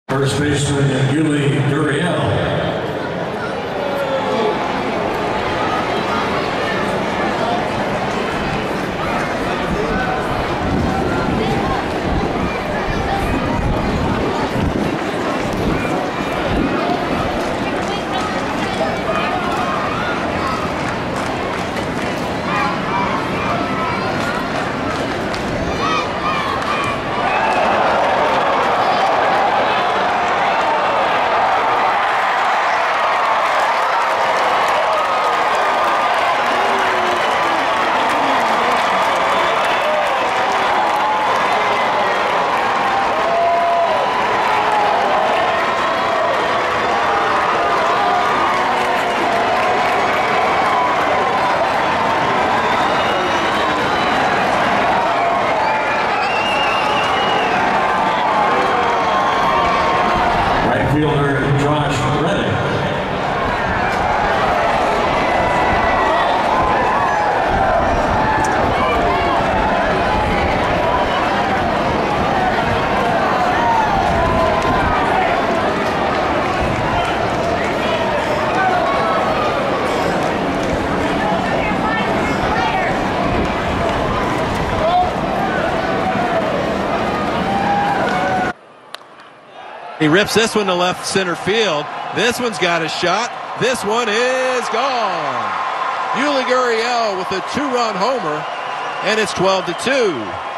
Rangers/Astros 7/14/19: Gurriel Homers for 10-Run Lead
Yuli Gurriel cranks the first pitch of the at-bat into the Astros' bullpen, a 2-run homer to make it 12-2 Houston.